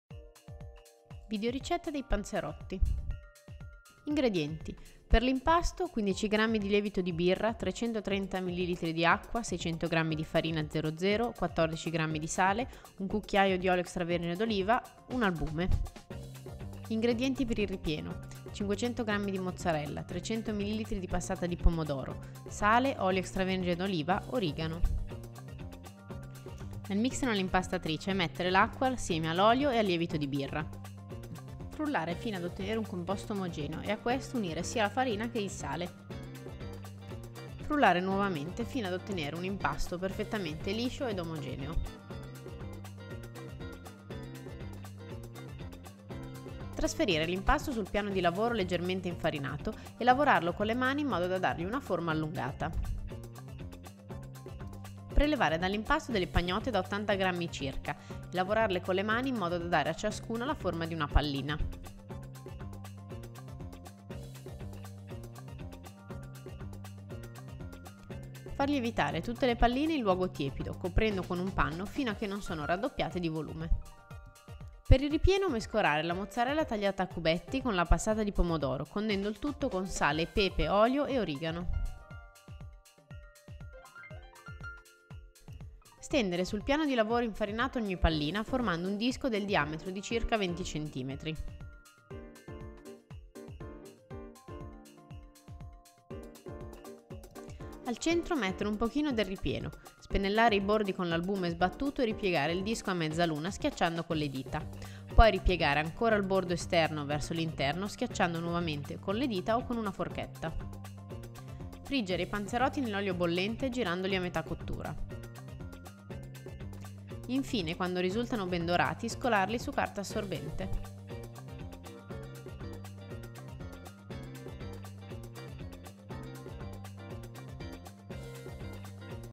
Panzerotti
Tipici della cucina italiana, i panzerotti sono dei piccoli calzoni preparati con un impasto lievitato e farciti a piacere, con mozzarella, pomodoro e tutti gli ingredienti che si possono utilizzare per farcire la classica pizza.

I panzerotti sono dei prodotti gastronomici tipici del sud Italia che richiedono la frittura come cottura; in base alle zone in cui vengono prodotti possono avere nomi diversi, come ad esempio pizza fritta a Napoli, calzoni fritti in tutta la zona della Sicilia dove è anche molto diffuso il pitone